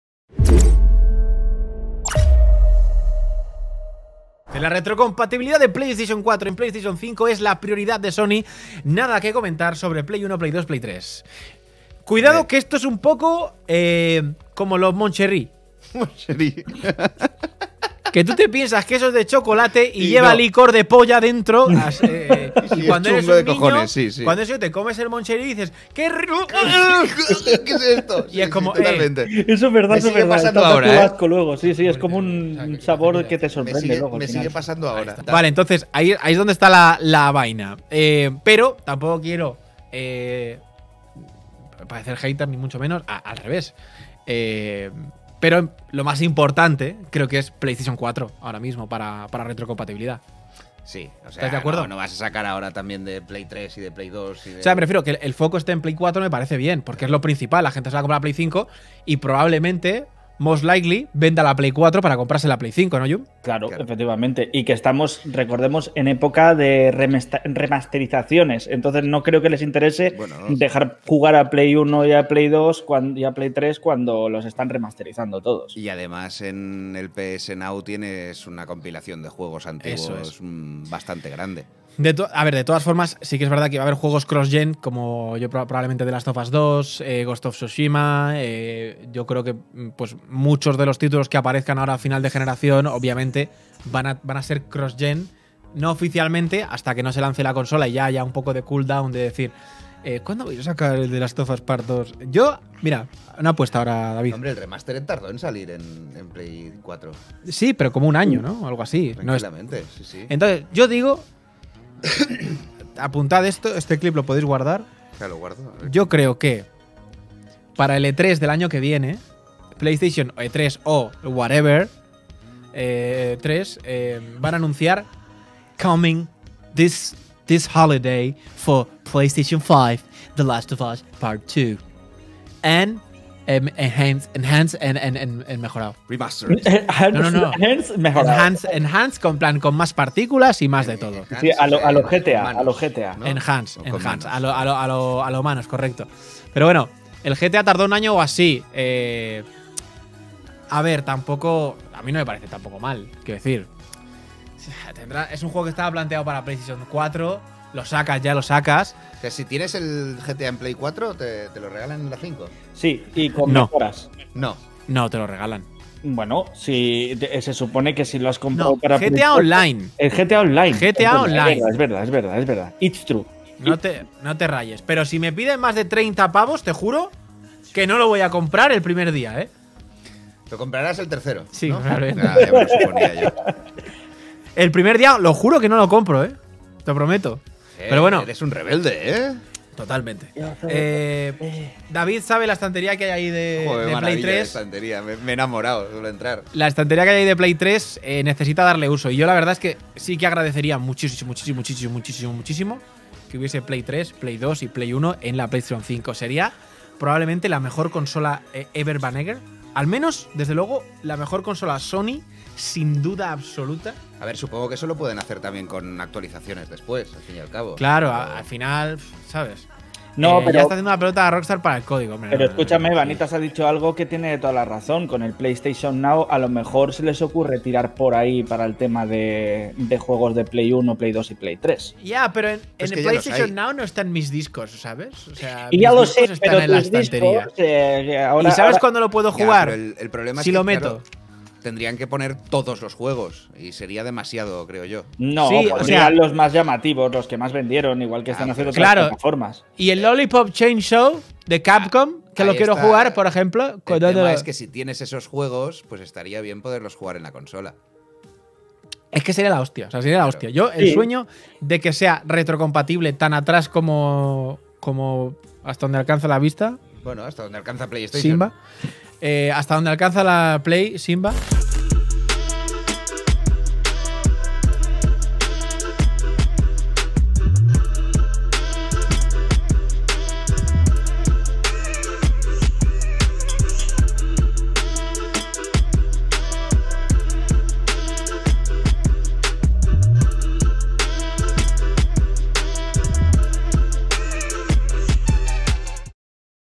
¿HABRÁ RETROCOMPATIBILIDAD EN PS5?| FUNNY GAMES MOMENTS
Sony ha dicho que PS5 permitirá jugar a tus juegos de PS4 pero no se pronuncia con anteriores generaciones.¿Será el adiós a lo retro y tener que comprar todos los remasters?

Nos gustan los juegos y la pizza